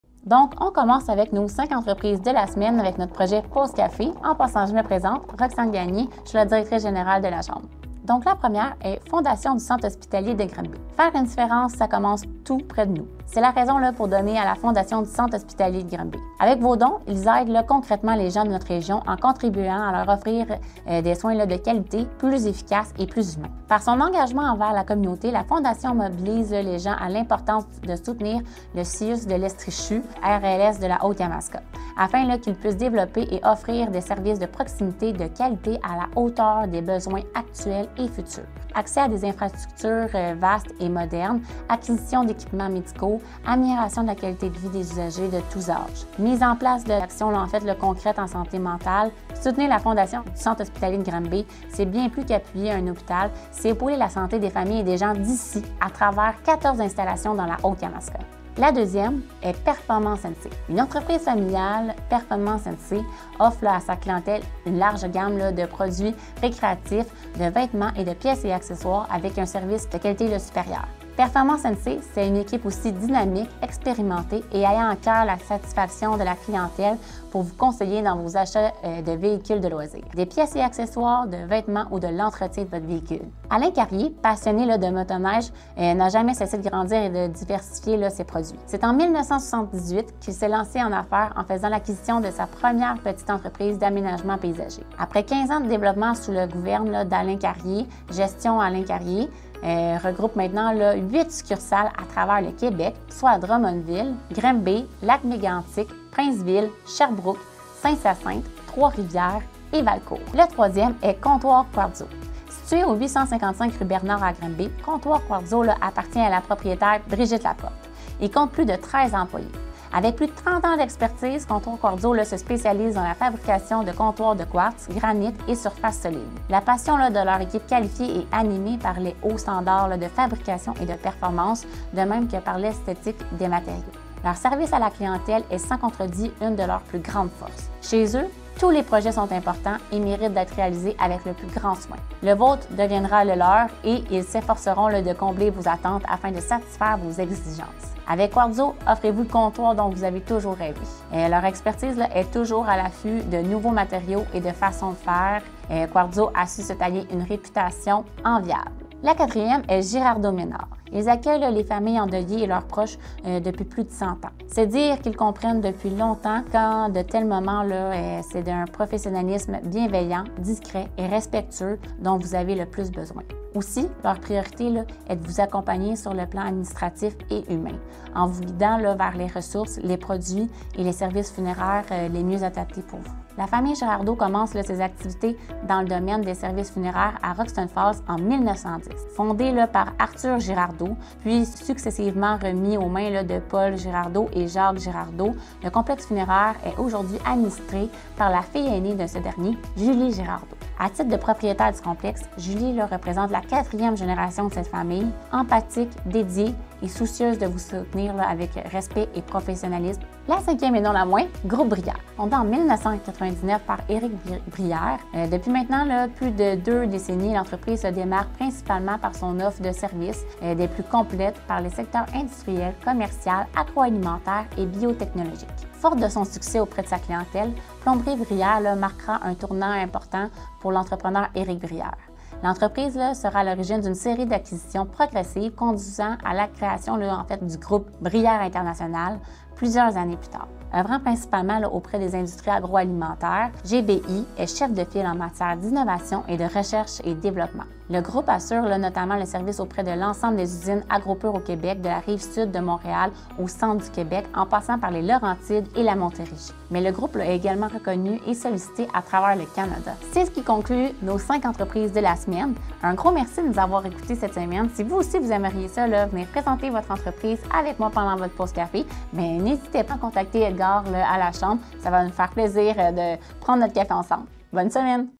Pause Café avec la DG de votre chambre - Semaine 16
Découvrez nos entreprises locales toutes les semaines avec Roxanne Gagné, directrice générale